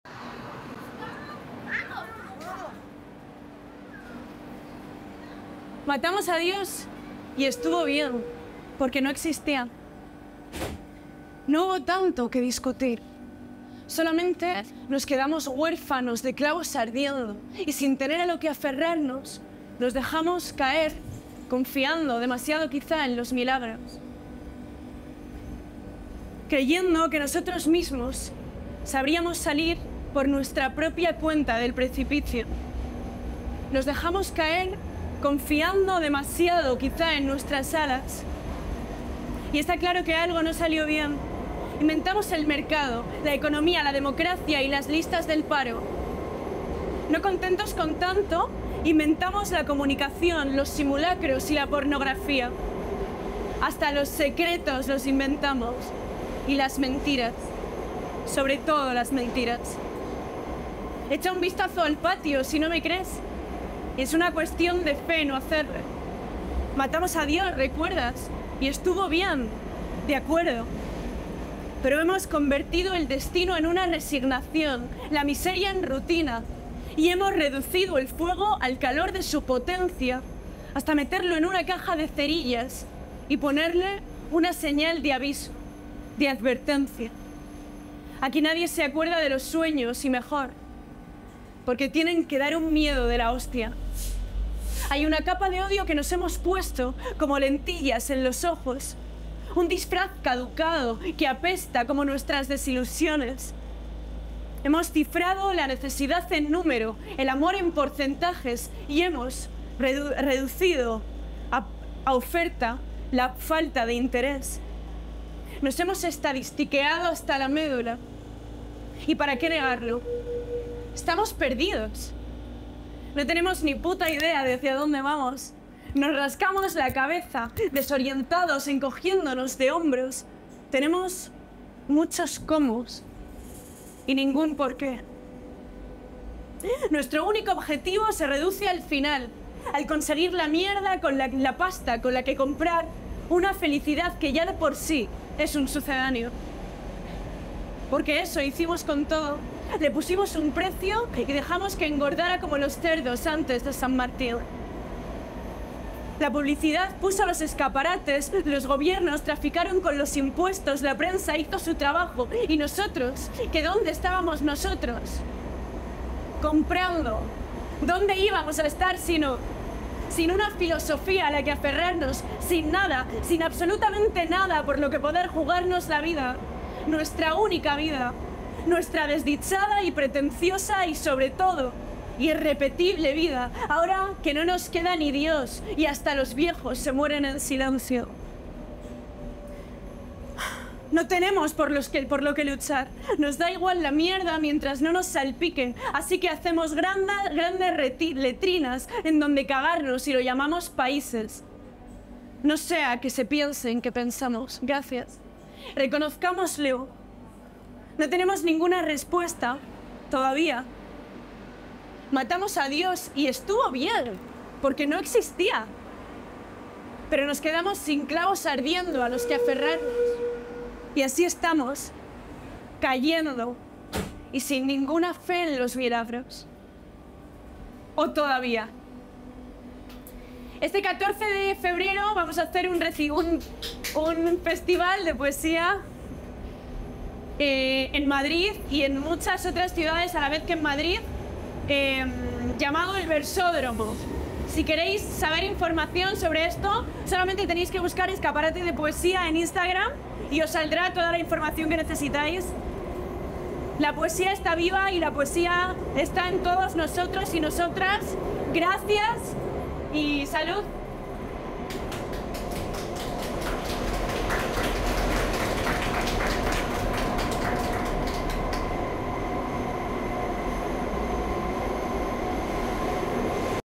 ARANTXA VÁZQUEZ - O Todavía (de Escandar Algeet) | Poesía en el Metro - Versódromo 2024
Poesía recitada en el Metro de Madrid (línea 6) para promover el Versódromo 2024. Para dar visibilidad al Versódromo, el colectivo Escaparate de Poesía junto con algunxs de lxs poetas que participan en el Festival, hemos llevado a cabo una serie de acciones. Entre estas acciones ha estado recitar poesía en el Metro de Madrid.

El 14 de septiembre de 2024 celebramos el Versódromo simultáneamente en Madrid, Granada, Sevilla, Barcelona, Bata (Guinea Ecuatorial), Lisboa, Milán, Ciudad de México, Bogotá, Miami, Santiago de Chile y Córdoba (Arg).

Grabación y edición: Itza Mendy